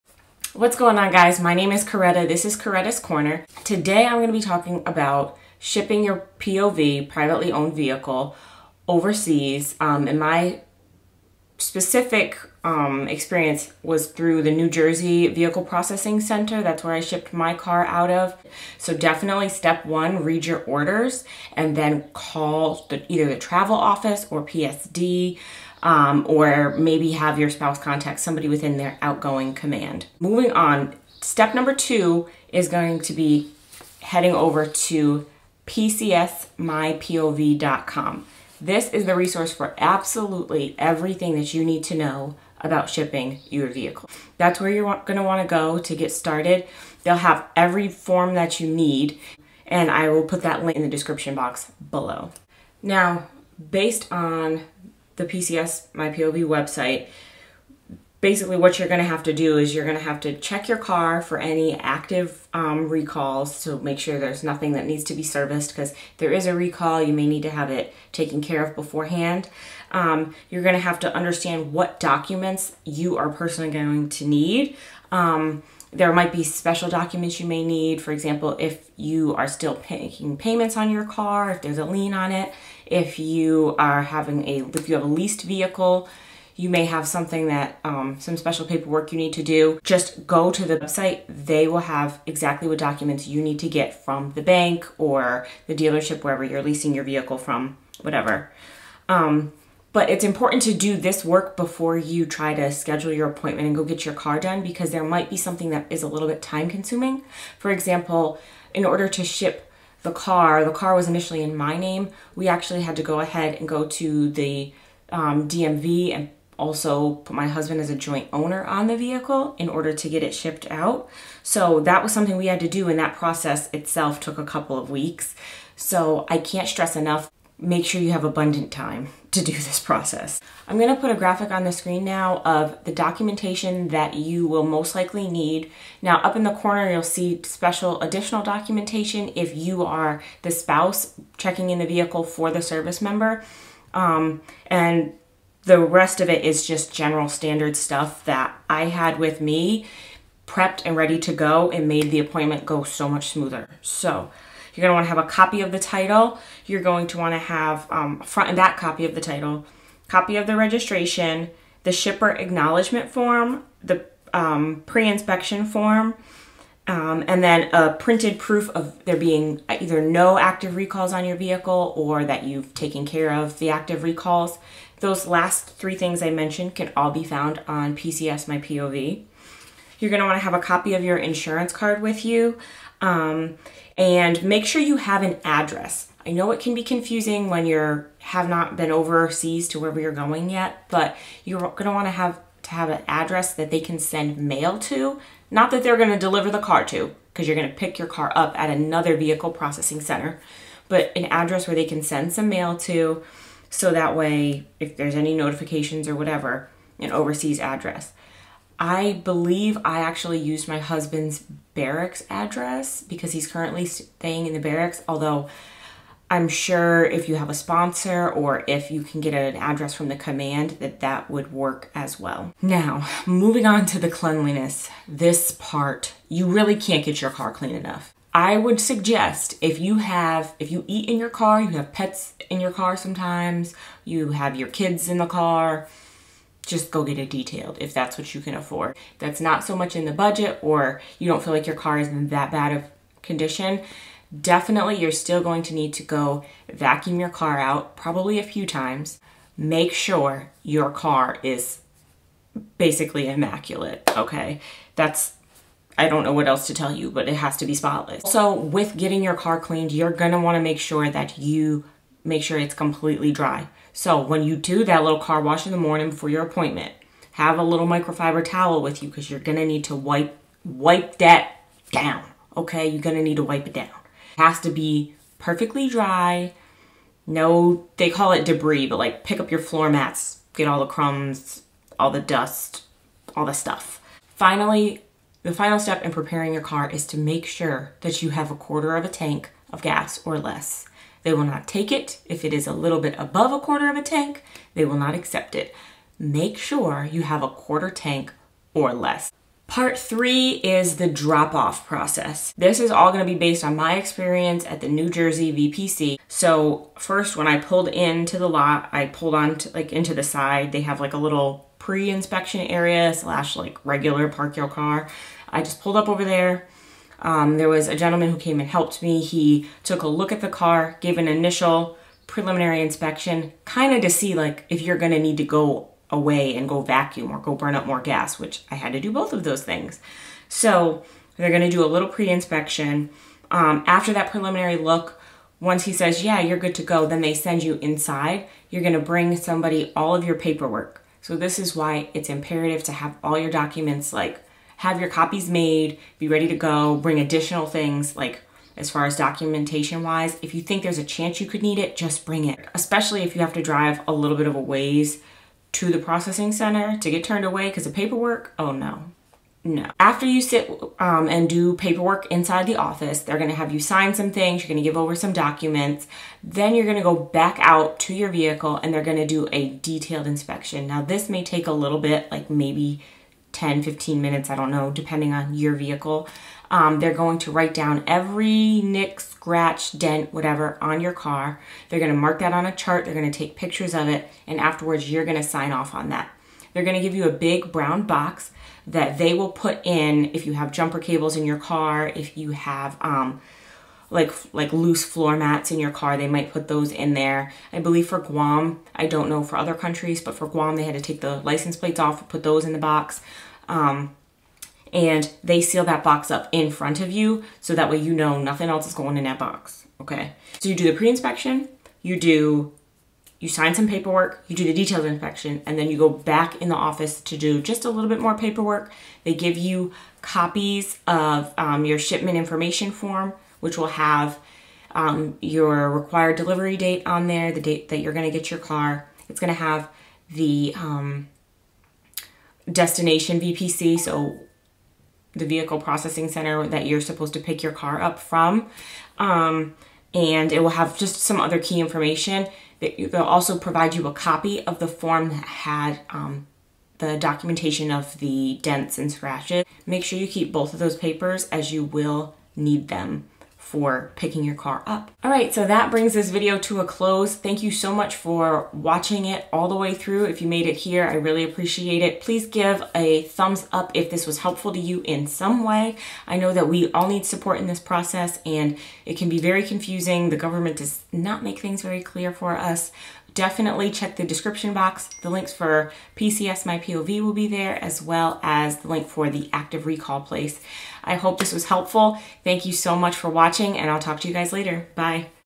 HOW TO SHIP A VEHICLE || OVERSEAS PCS || STEP BY STEP TALK THRU (Military Spouse)
civilian spouse ships car for overseas PCS. This is my experience shipping our car from the New Jersey VPC to Agana, Guam for our upcoming military move.